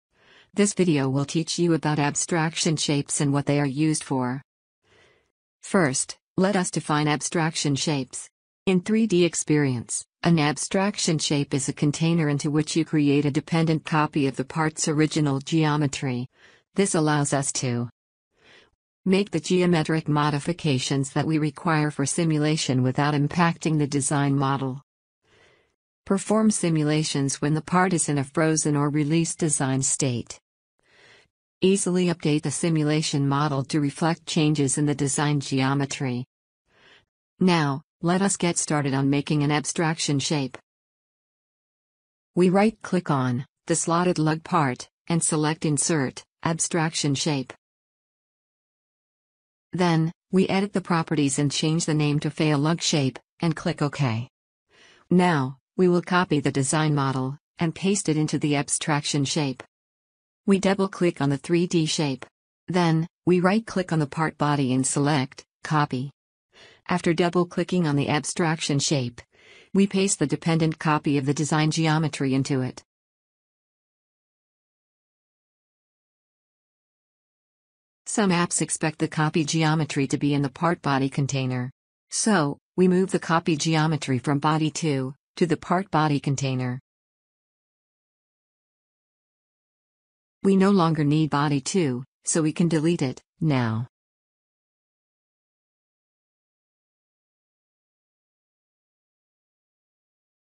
Create an Abstraction Shape in 3DEXPERIENCE
A short video showing how to create a dependent copy of the design model for simulation. In 3DEXPERIENCE, this is called an Abstraction Shape.